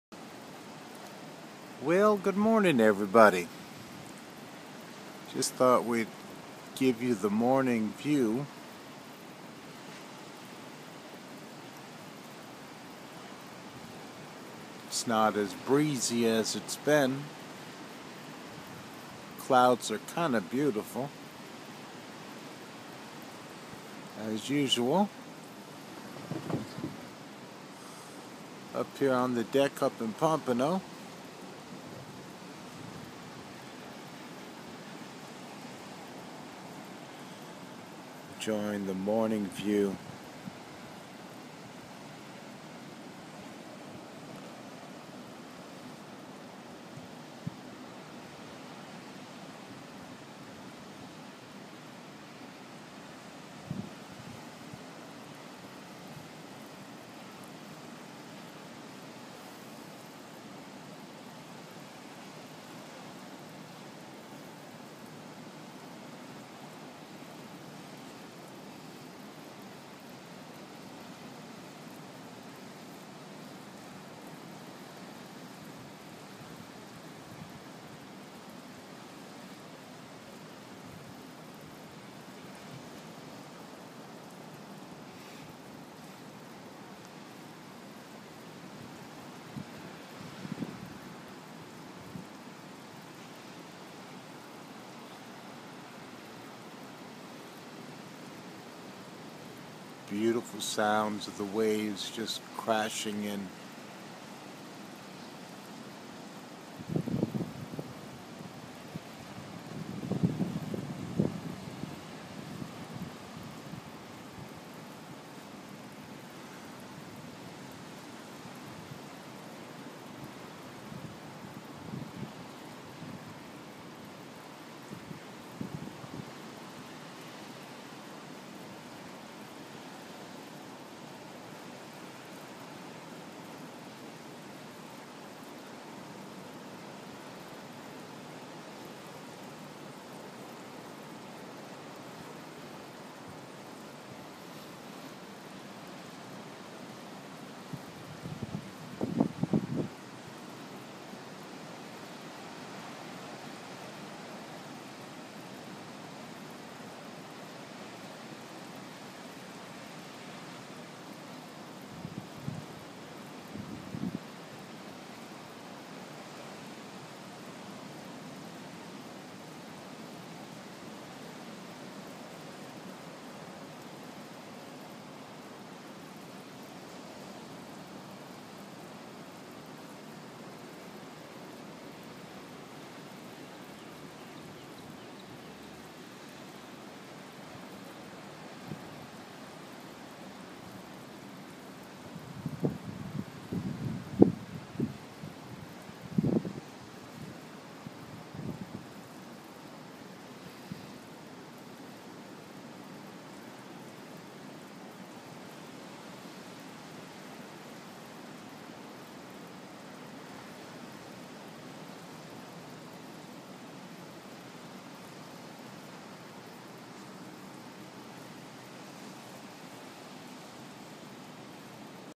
Ocean View after a stormy night.
A calming short clip of the beauty of the view of the Ocean and the sounds of the Wind and the Crashing of the Waves.